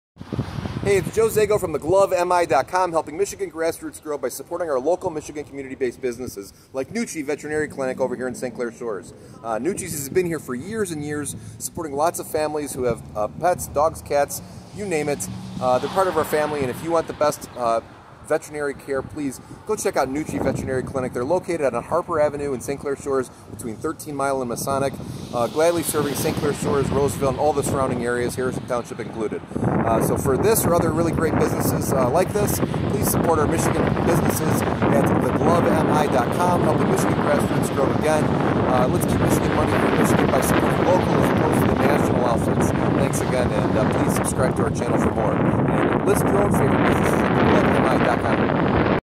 TheGloveMI.com recommends Nucci Veterinary Clinic in St Clair Shores
TheGloveMI loves Nucci Vet Clinic to help with allof your pets needs to stay healthy. Our dogs and cats our part of the family and so is Nucci as they have been here supporting to=housands of local St Clair SHores area pet owners for decades. Located on Harper Ave just north of 13 Mile Road, Nucci is centrally located and very eas to find.
Help support great local community based Micgigan cusinesses like Nucci Veterinary Clinic in Saint Clair Shores. and see how you can help support other local businesses.